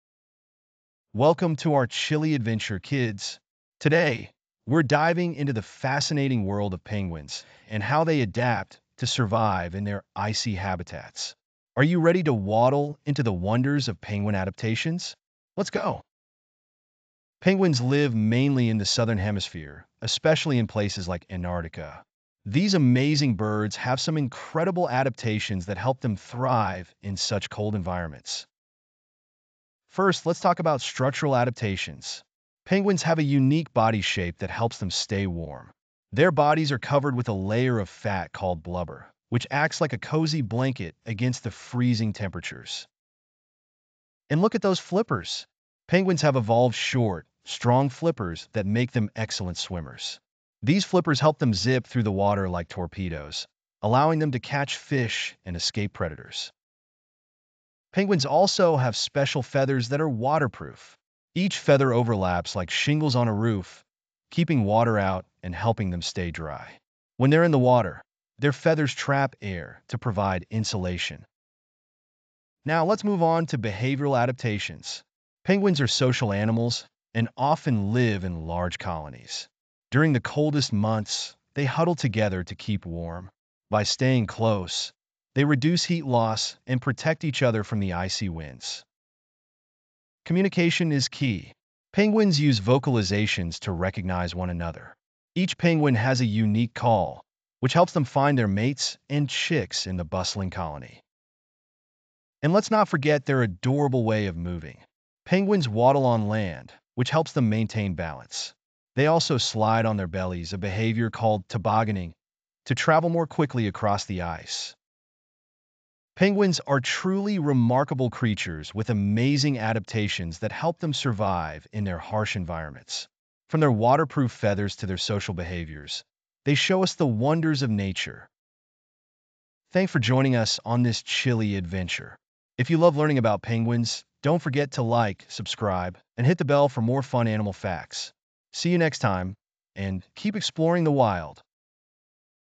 Adaptation In Penguins | Adaptation In Animals | Science For Kids | Science Adaptation For Grade 4
Adaptation In Penguins | Adaptation In Animals | Science For Kids | Science Adaptation For Grade 4: In this video we will know more about Penguin's habitat, stractural adaptation and behavioural adaptation of penguin.
Join us on a chilly adventure exploring the fascinating adaptations of penguins that enable them to thrive in icy habitats. Discover their unique body shape, blubber layer, waterproof feathers, and strong flippers that make them excellent swimmers. Learn about their social behaviors, including huddling for warmth and unique vocalizations. Penguins showcase the wonders of nature with their remarkable survival strategies!